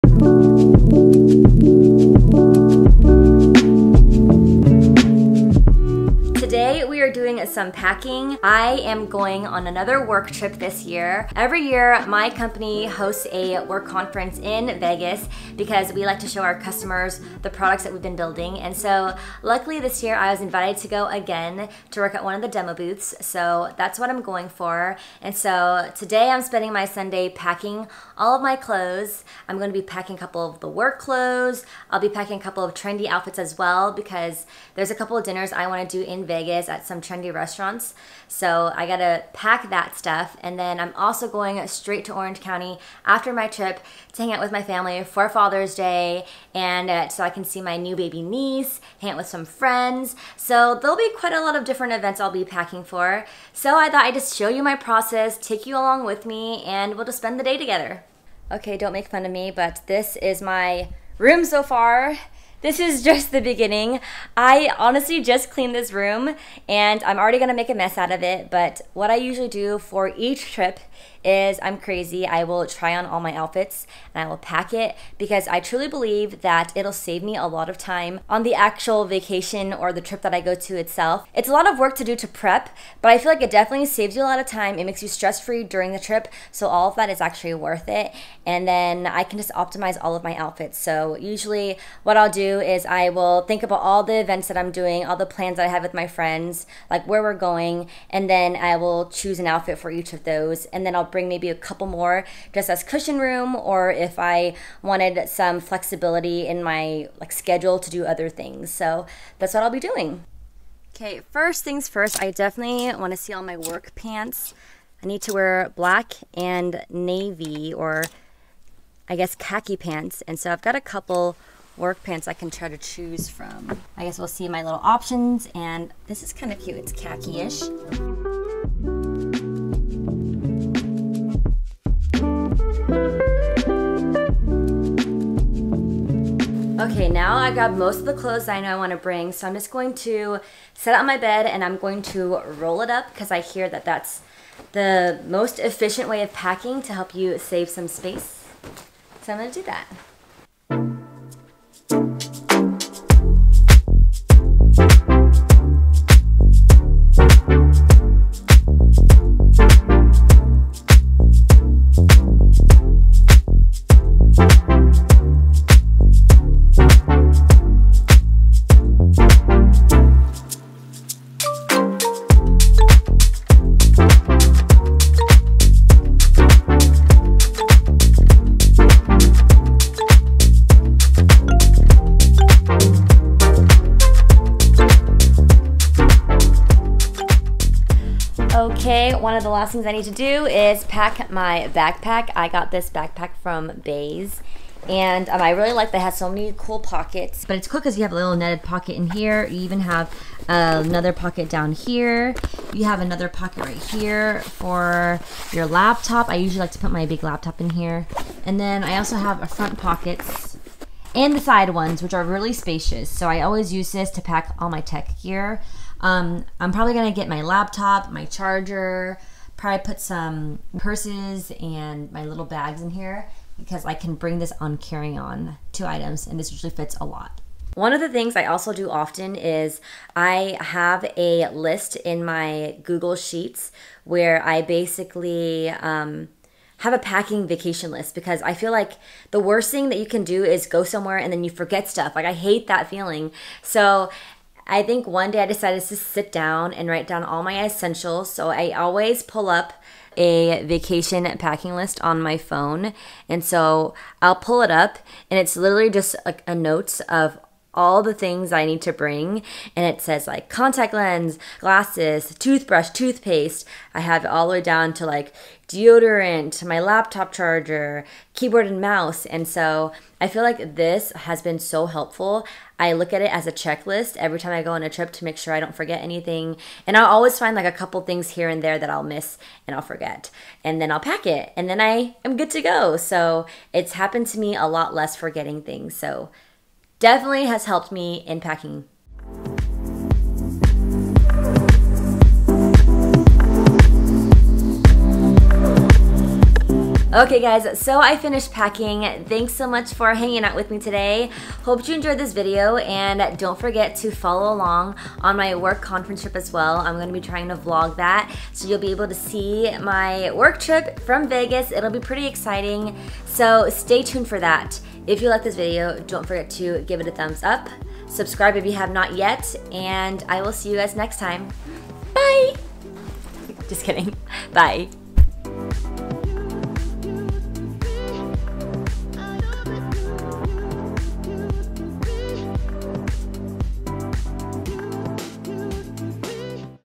Pack with Me for a Work Trip
Spend the day packing with me for a work trip to Las Vegas